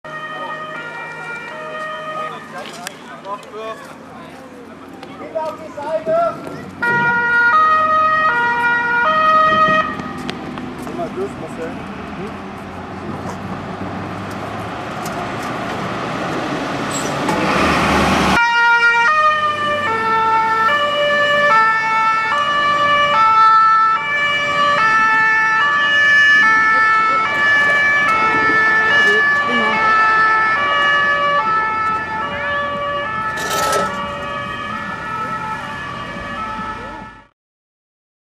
[Abrücken zum Einsatz von der Jubiläumsfeier] DLK 23/12 der Berufsfeuerwehr Frankfurt BLW 3
Eine DLK 23/12 der Berufsfeuerwehr Frankfurt am Main rückt von der Jubiläumsfeier der FF Sindlingen zu einem Realeinsatz aus. Das Fahrzeug ist auf der Bereichsleitungswache 3 in Frankfurt-Nied stationiert.

Gefilmt in Frankfurt am Main 08.2017